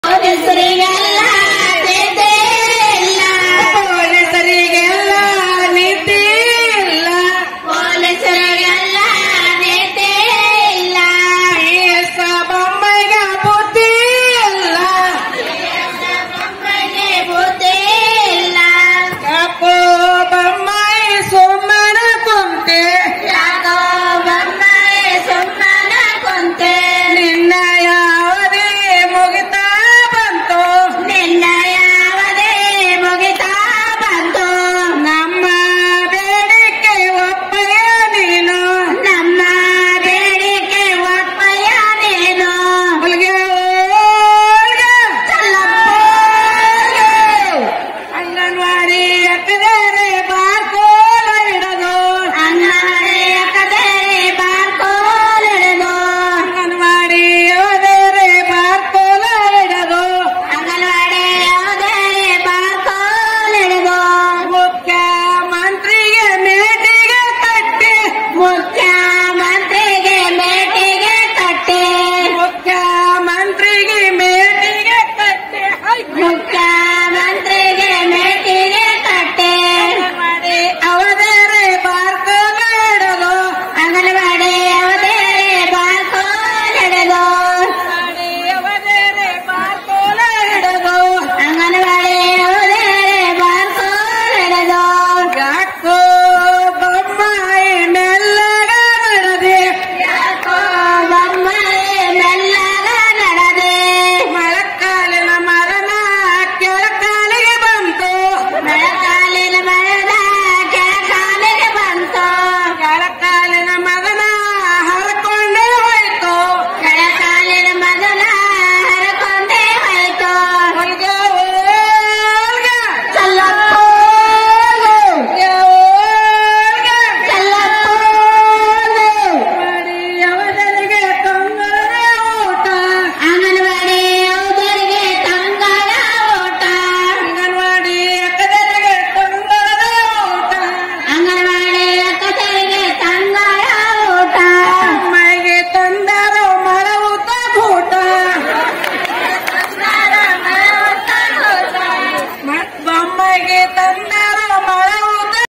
A Folk Song/ ಬಿಸಿಯೂಟ, ಅಂಗನವಾಡಿ ಕಾರ್ಯಕರ್ತರ ಹೋರಾಟದಲ್ಲಿ ಜನಪದ ಹಾಡು
ಅಂಗನವಾಡಿ ಕಾರ್ಯಕರ್ತೆಯರೂ ಸೇರಿದಂತೆ ವಿವಿಧ ವರ್ಗಗಳ ಶ್ರಮಿಕ ಕಾರ್ಮಿಕರು  ಬೇಡಿಕೆಗಳ ಈಡೇರಿಕೆಗೆ ಆಗ್ರಹಿಸಿ  ಬೆಂಗಳೂರಿನಲ್ಲಿ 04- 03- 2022ರ ಶುಕ್ರವಾರ ಮತ್ತು ಶನಿವಾರ ಪ್ರತಿಭಟನೆ ನಡೆಸಿದರು. ಶನಿವಾರದ  ಹೋರಾಟದಲ್ಲಿ ಕೇಳಿಬಂದ ಜನಪದ ಶೈಲಿಯ ಹಾಡಿನ ನೋಟ/
Different sections of labour workers, including Anganwadi activists staged a protest on Friday and Saturday in Bangalore on 04 & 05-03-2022,  demanding the fulfillment of their demands. Folk style song heard during Saturday's protest.
Don't miss following links;